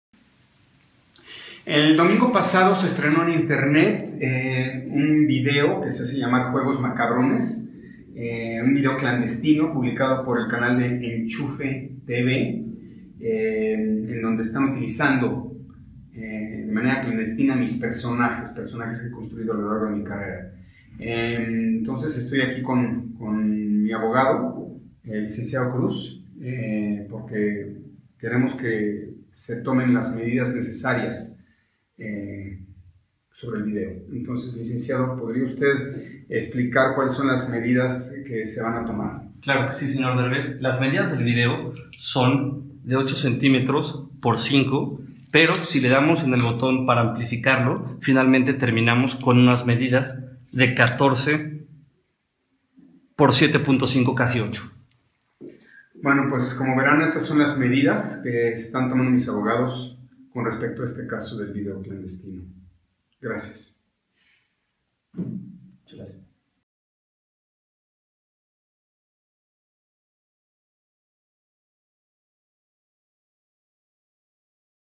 Eugenio Derbez SE MOLESTA😡😂& le reclama A EnchufeTV por copear un personaje de EL😅😏
MIRA HASTA EL FINAL😑